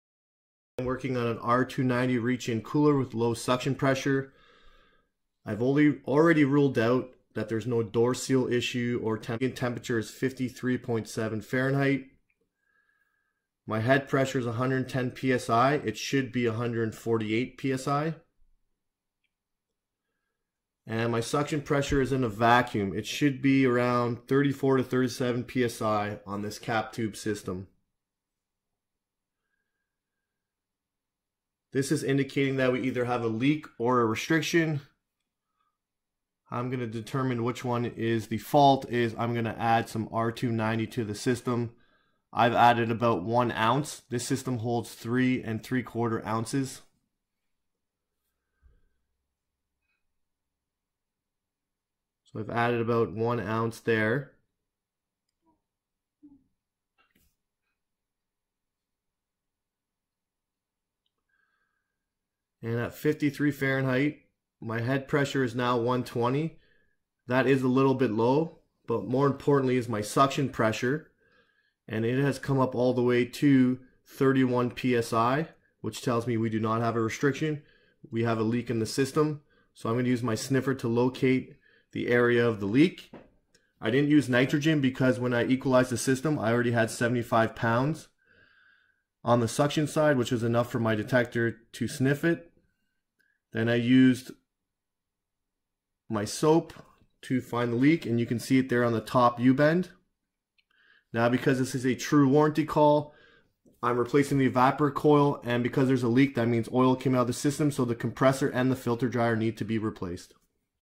True R290 Cooler has A Frozen Evaporator Coil And is not Getting Cold Enough.
In this video, I will troubleshoot a frozen evaporator coil on an R290 system and will find that the unit is low on charge and will find the leak via the electronic leak detector.